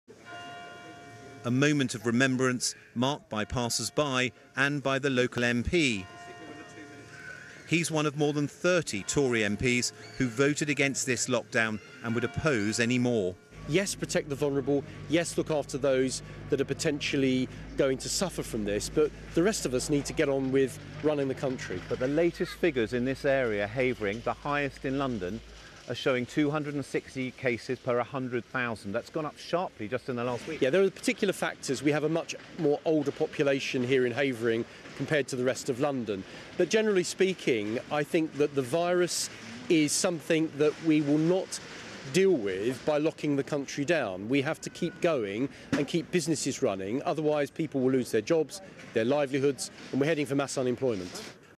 Andrew Rosindell Discusses Lockdown at Romford Market
On the 11th of November, BBC London broadcast a short interview with Andrew Rosindell from Romford Market in his constituency, discussing lockdown.

Andrew explained to the BBC's Tim Donovan why, in his view, the country needs to get moving again while doing all it can to protect the vulnerable from COVID-19.

The interview was part of a segment which included a range of views on lockdown and coronavirus restrictions.